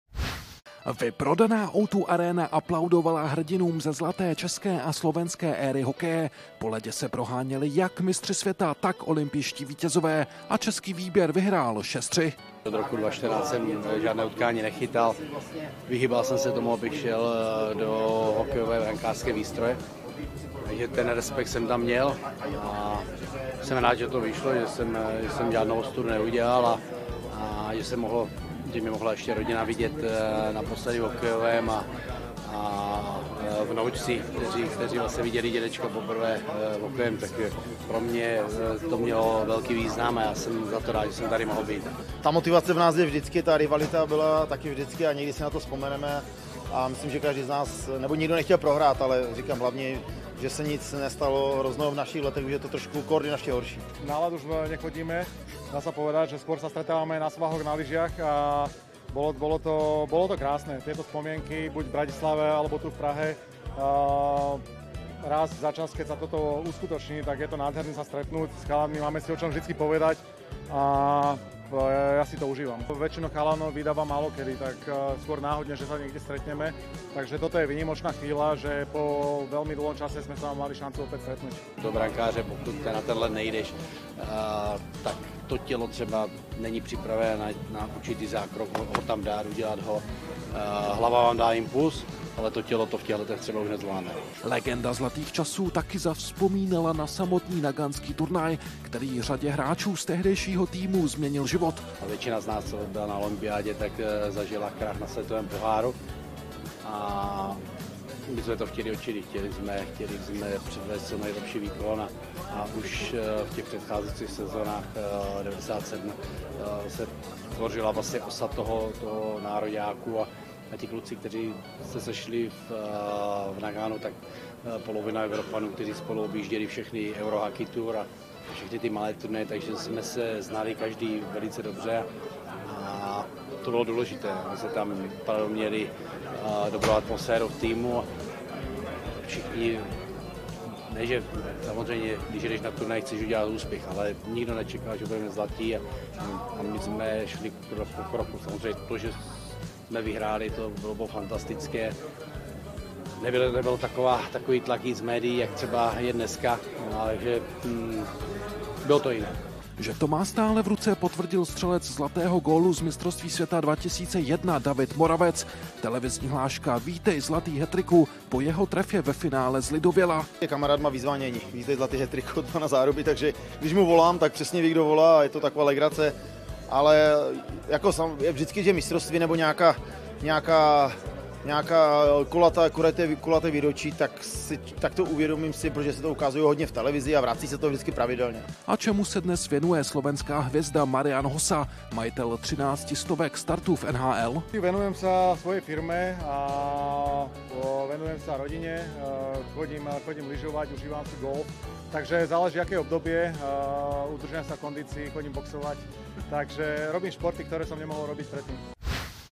Vyprodáno i na „důchodce”. Nechytal jsem devět let, říkal Čechmánek
Exhibice v O2 areně při příležitostí 25. výročí od vítězství v Naganu byla legendární!